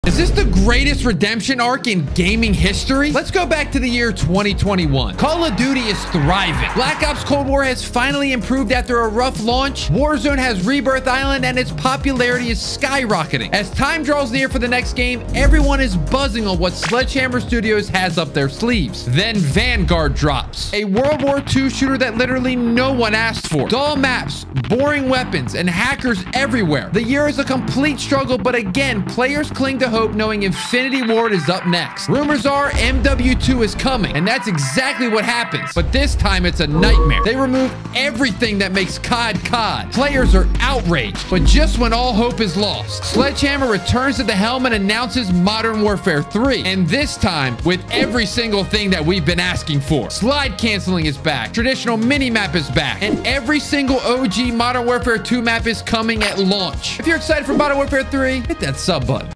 Call of Duty did something good?
Call of Duty is doing the unthinkable with MW3….LISTENING. the question is, are you excited?